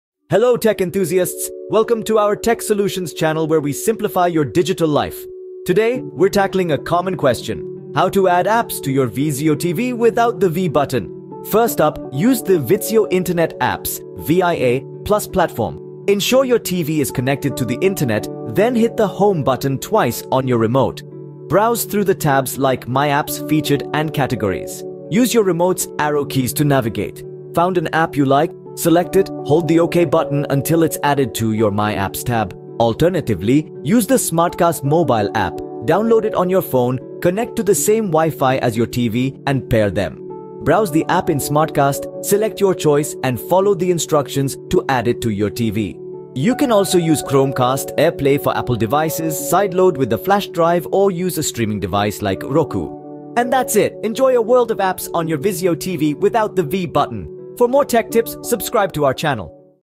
How To Add Apps To Vizio Tv Without V Button
In this informative tutorial video, we will guide you step-by-step on how to add apps to your Vizio TV without using the V button. No more struggling or searching through menus! We will provide you with tried and tested methods to make the process hassle-free and straightforward.

Whether your V button is not working or you simply prefer alternative methods, we've got you covered. Our experts have gathered all the necessary information to ensure you can easily install your favorite apps on your Vizio TV.

From utilizing the Vizio remote app to accessing the SmartCast feature and even using external devices, we will explore various techniques that will give you immediate access to a wide range of apps.

So, if you're looking for a reliable and proven way to expand your app library on your Vizio TV, look no further! Join us in this tutorial and learn how to seamlessly add apps without the V button.

Let's empower every Vizio TV owner with the knowledge they need to enhance their viewing experience!

OUTLINE:

00:00:00 Introduction
00:00:11 Using Vizio Internet Apps (VIA) Plus platform
00:00:32 Using the SmartCast Mobile App
00:00:46 Other methods to add apps
00:00:53 Conclusion